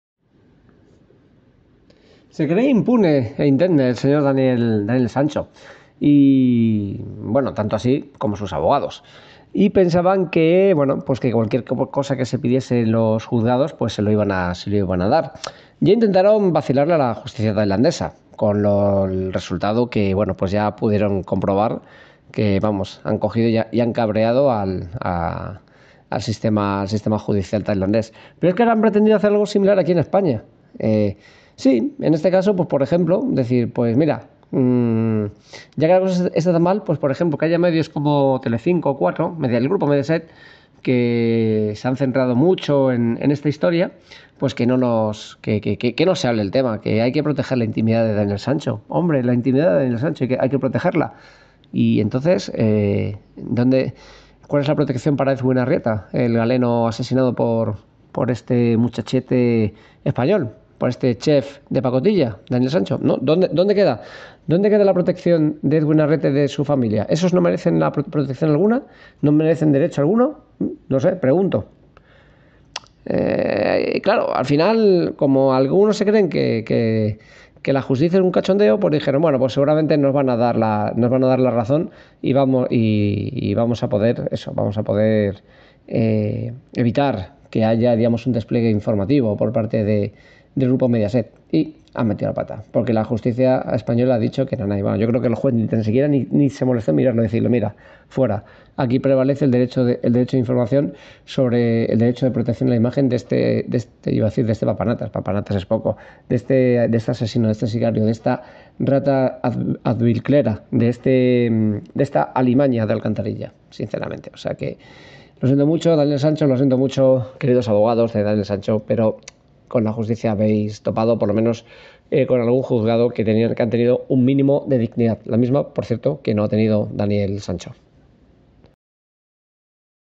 La Justicia se harta de los caprichos del asesino de Daniel Sancho y le advierte que ni una más
Al chef se le acabó el chulearse de la Justicia y de los familiares de Edwin Arrieta.